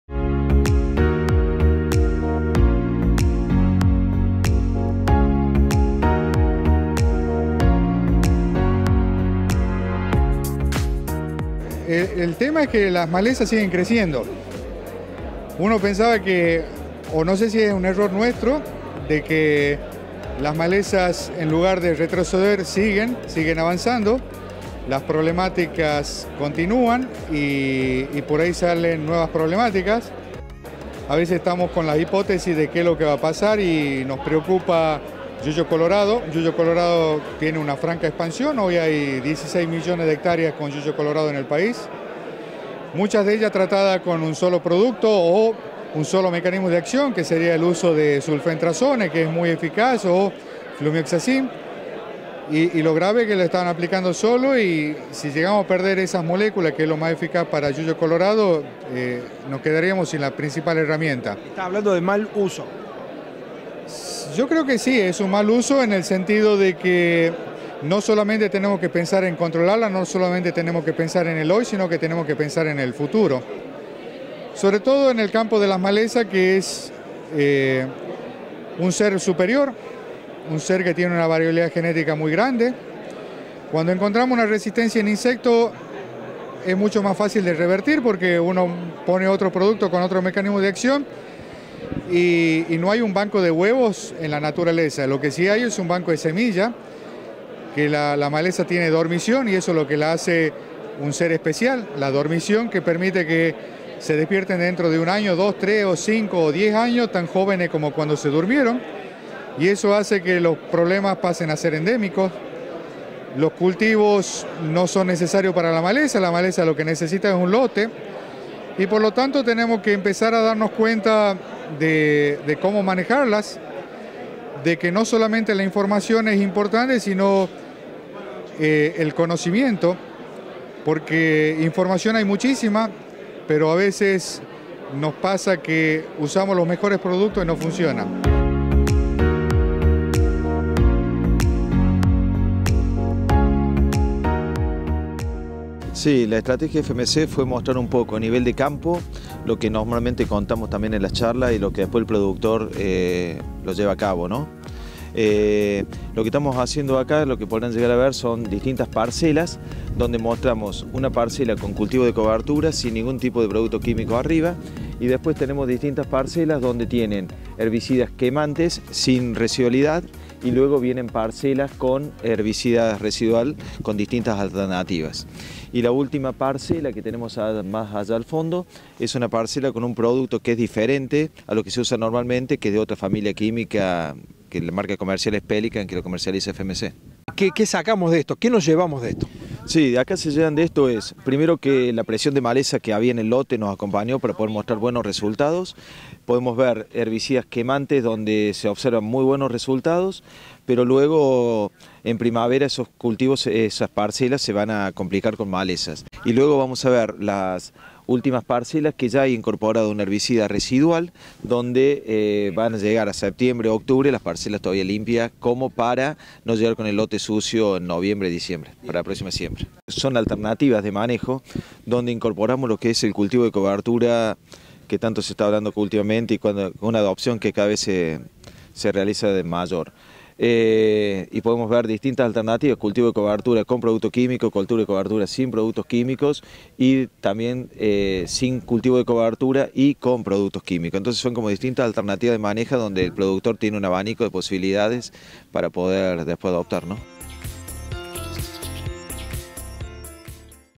15to Encuentro de Monitoreo 3ra parte
15º Encuentro Nacional de Monitoreo”, comenzamos a revivir los mejores momentos del mayor evento técnico del país.Fuente: